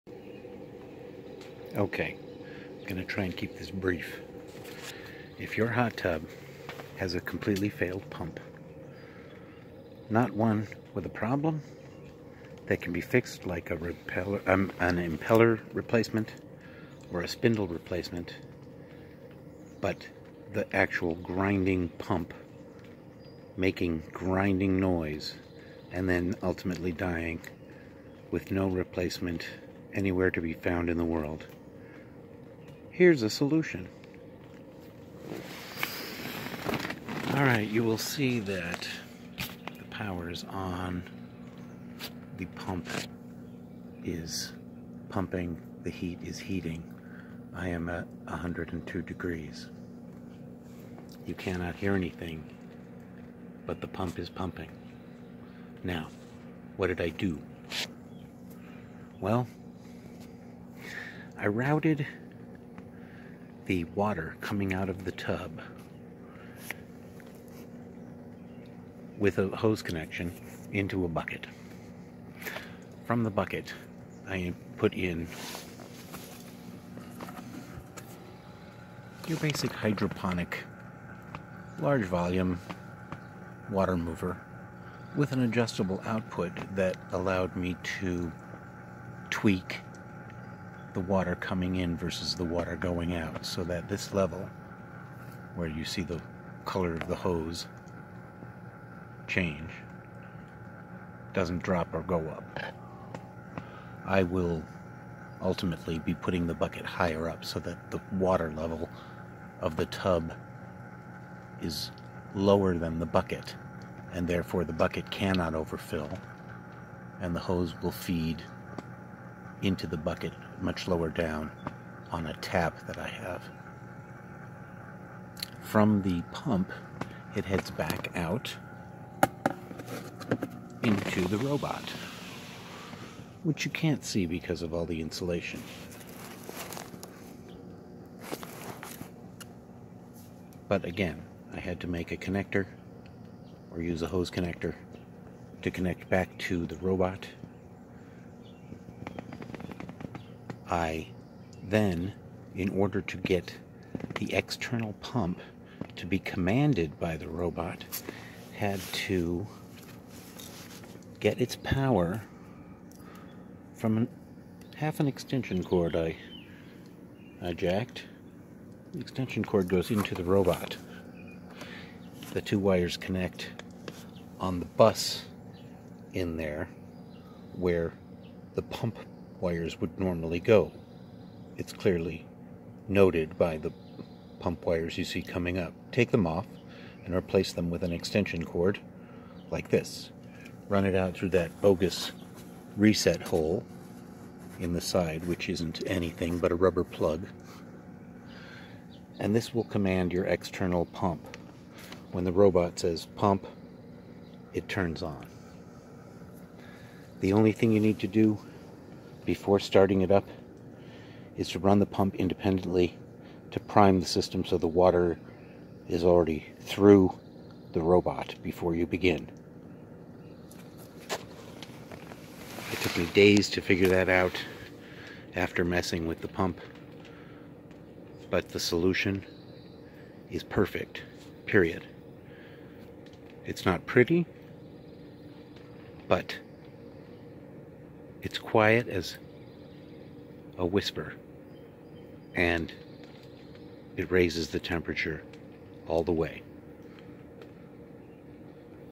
Pump Bypass (Saluspa/Lay Z spa/ Bestway/Intex/Coleman) solution.
This may be a bit McGuyverie for some, but this company does not provide a replacement pump. You can get impeller and the spindle, but the pump if the bearings go bad renders the pump useless. during the covid-19 down time I was forced to be creative. I would normally have just ordered the pump and replaced it, but they do not provide that option. Given that I am a handyman with lots of materials around I was able to rig this system. the important components a submersible pump that moves at least 350 gallons per hour. I had one from an old Hydroponics failure, but it was about 35 bucks online. You need a bucket and a way to elevate it. He might want a bucket cap the tap flow down on the bucket for the transference of water from the tub. you might want to cover for the bucket oh, and some insulation like seen in the video or wrapped and taped or pieces of swimming pool noodle would work well. the heart return is connected directly to the tub. I have forfeited the air blower because we do not use it. it provides enough movement for the tub Outlet to bend away and the robot Inlet tube and Away both to go to the reservoir. you will want to insulate the reservoir as well as the hoses. currently mine is in double paper bags but because I have excess are 20, I will wrap everything and tape. the pieces of hose and appropriate hose connectors are easily acquired from a hardware store. I had some of the clear hose that fit the pump from my hydroponics, and I snipped a piece of retired hose to makethe tub outlet hose.
You would like to simply pop the electric submersible directly into the tub, but that bypasses the filters. it kept popping into my head why don't you just put the pump in the tub and I kept forgetting the filtration. The only way this works is if the pump is in a Reservoir outside the tub.
UPDATE: the pump can run continuously.
There is no real need to hotwire an extension chord.